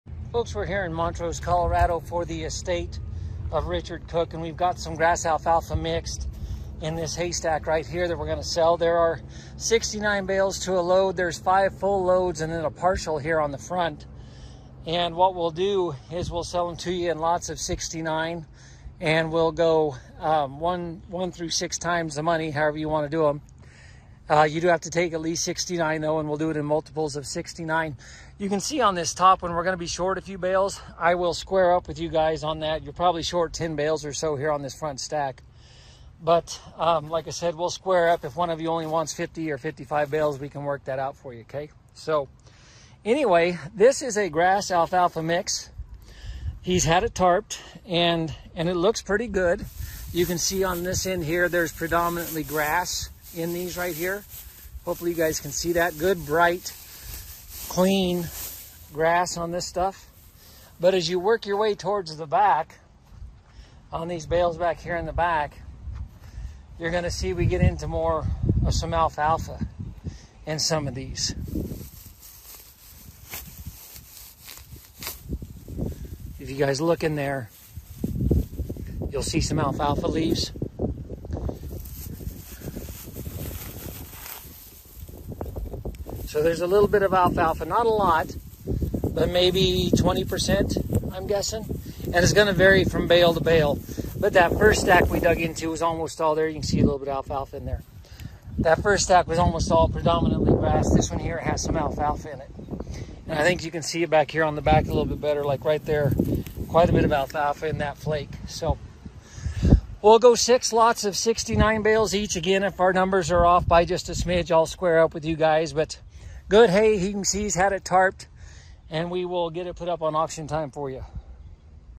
For Sale
This is the "Middle" stack and we may have to adjust the final bale count on each lot dependent on total bales.

No sales tax on hay sales.  I think this is 2022 second cutting but I can't guaranty that.  It is predominantly grass with a little alfalfa mixed in - I'm guessing 80/20.  Good hay that has been tarped since it was put up.  Each balewagon load is 69 bales so we are going to sell it that way - in lots of 69.

No loading assistance is available so you will need to plan on loading it yourself and we need it picked up between October 22 and October 29.  We can accept credit card payments on hay purchases with a 4% processing fee.  Please call me with any questions - 970_210_6149.